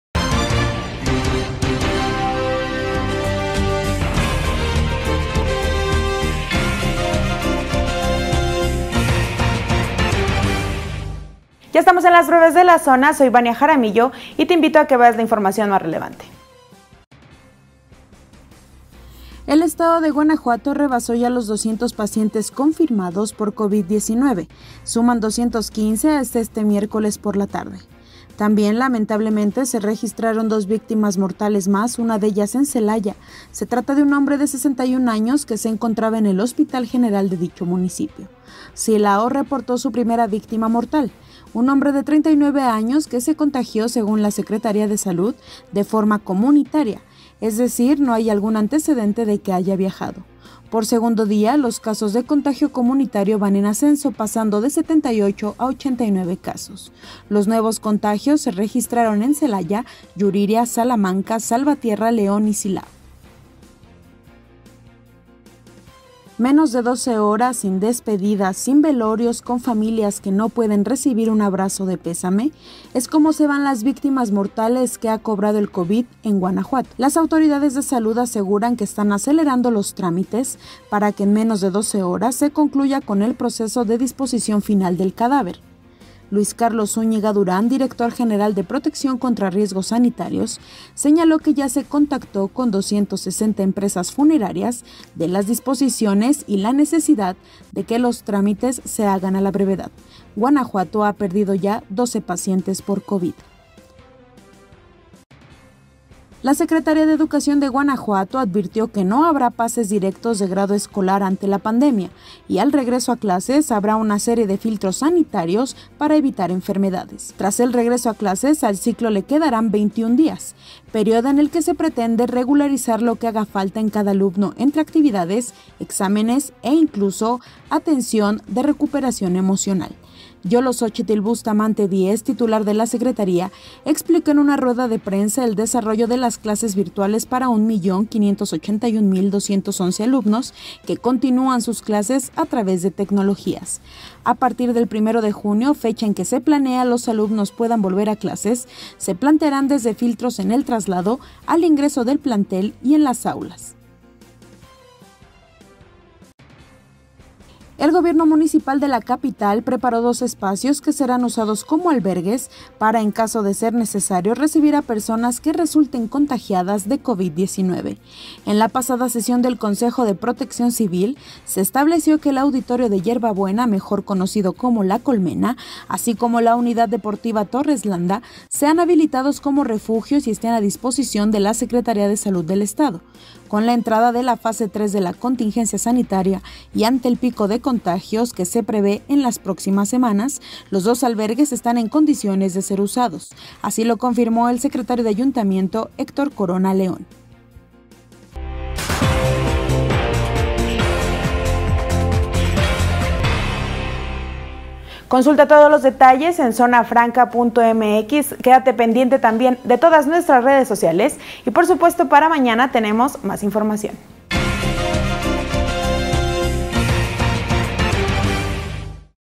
Supera Guanajuato 200 casos y suma 2 muertos más por COVID-19 | Breves de la Zona
- Alistan dos refugios para enfermos de COVID19 en GuanajuatoCapital.
- Las víctimas mortales por COVID-19 solo pueden ser incinerados o inhumados en Guanajuato.
- Supera Guanajuato los 200 casos, hay dos víctimas mortales más por COVID-19.